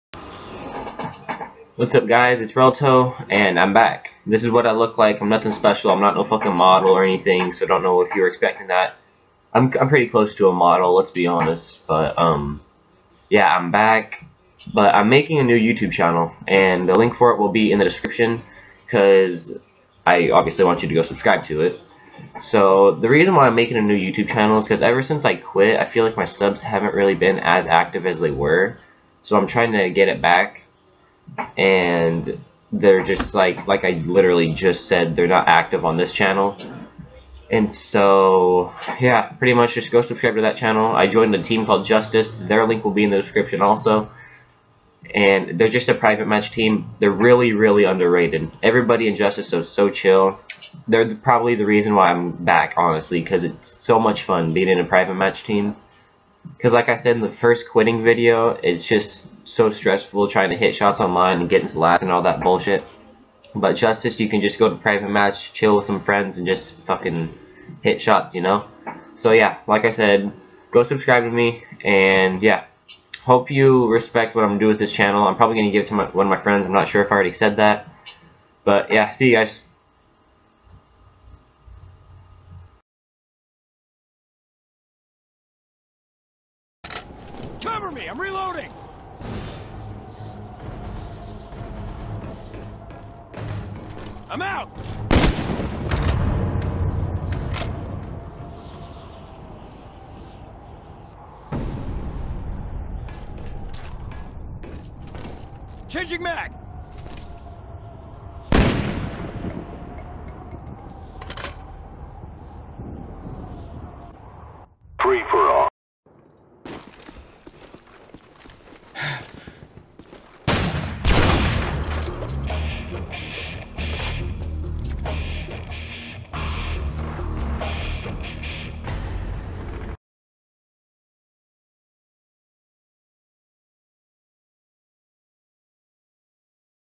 Face Reveal-Im Back?!- New Channel!!
YO GUYS WHATS UP!?
Ya boy is back, With Face Reveal, So badass right?
Well i made a new channel and if you wanna still be subbed to me and see what i come out with and hit in the future, Go Sub to my new channel!